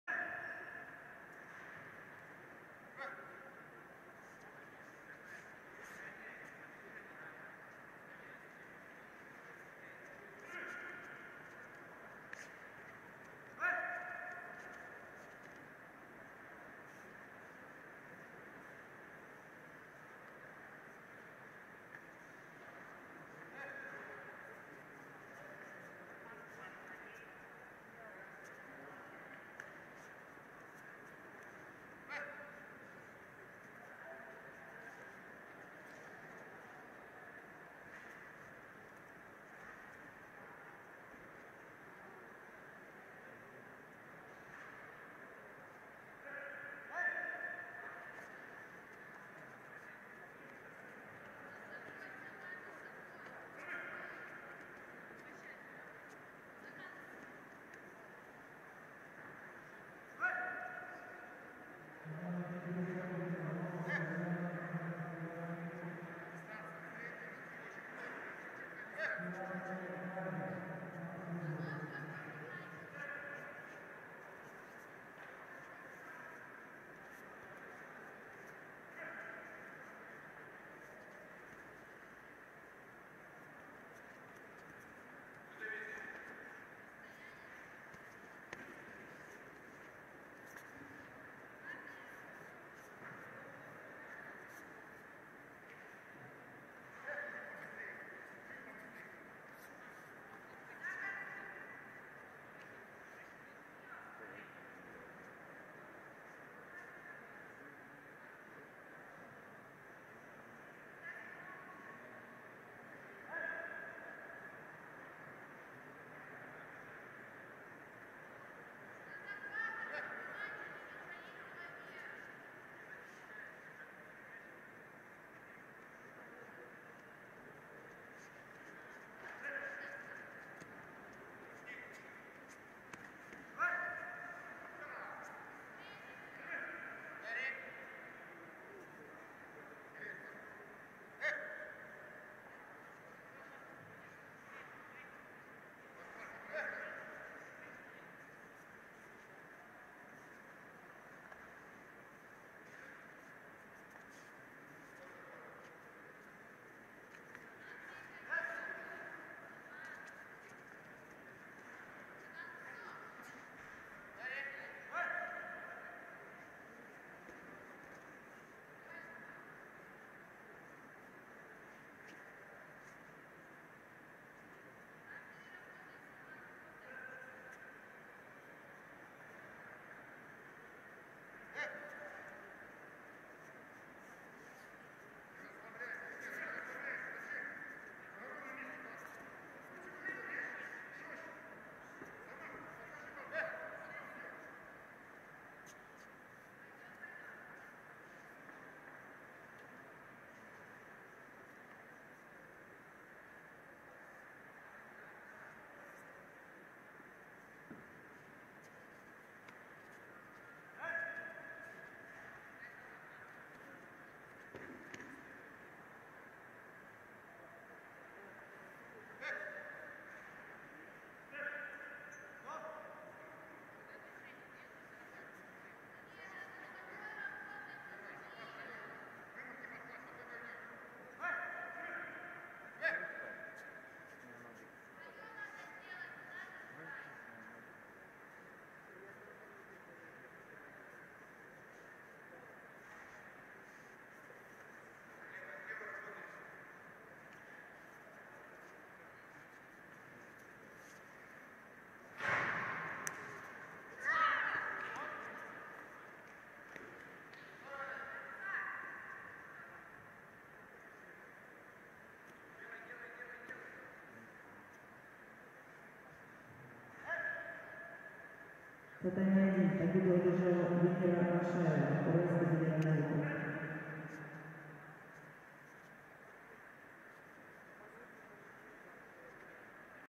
52 kg Чернышёва М Машаева В 18 06 2021 Т1 Первенство России среди юниоров и юниорок до 23 лет
Первенство России по дзюдо среди юниоров и юниорок до 23 лет

Место проведения: Россия, МУРМАНСК